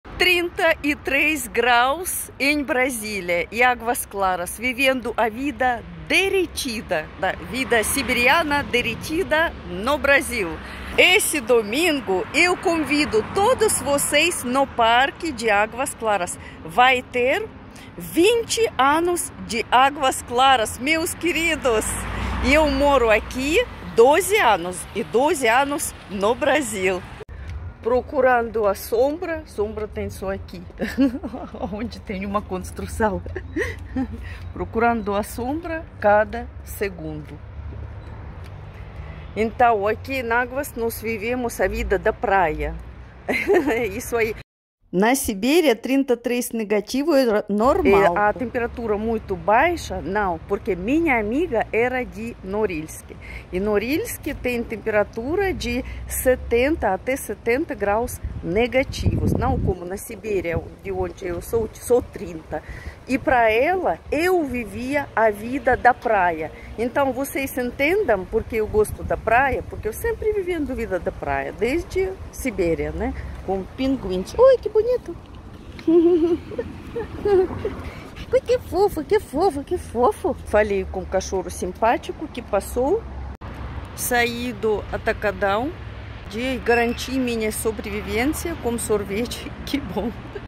CALOR NO BRASIL
A festa de 20 anos de Águas Claras DF no domingo!
No parque ecológico 🥳🥳🥳

Aguenta calorrrr de 33 graus e vem festejar 🥳🍻🍺